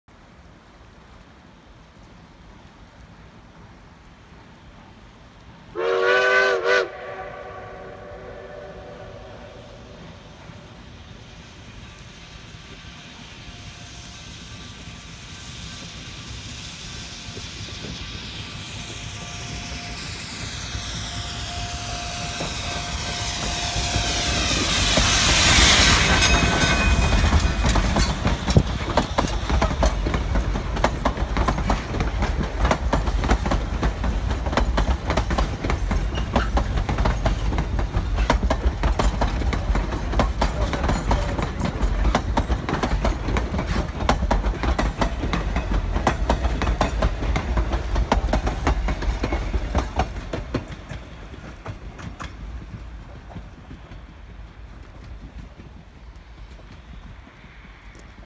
Durango & Silverton Railroad in UHD
Very short video (58sec) taken in Silverton on August 13, 2019. iPhone 4k video - apologies for automatic exposure.